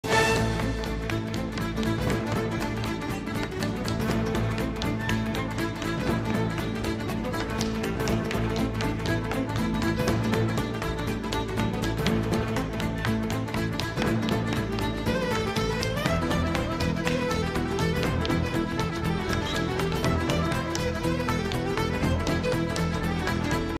The Incredible Dancing Irish Parrot
This black-headed parrot is a master of riverdance.